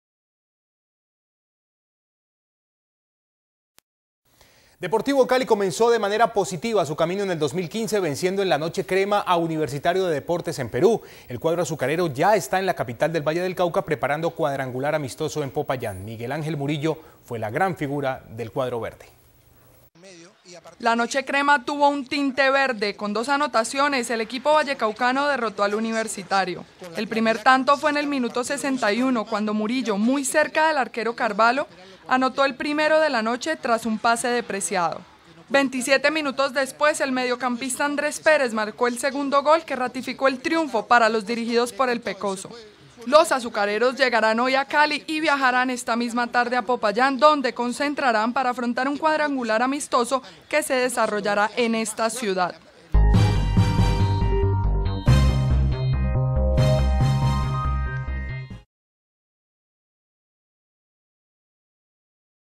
Enero 23 de 2015 Triunfo del Deportivo Cali ante Universitario en Lima
En una noche adornada por los peruanos, el Deportivo Cali fue el protagonista de la noche al quedarse con la victoria en su visita al país inca, con un 2-0 sobre Universitario de Deportes en la "Noche Crema".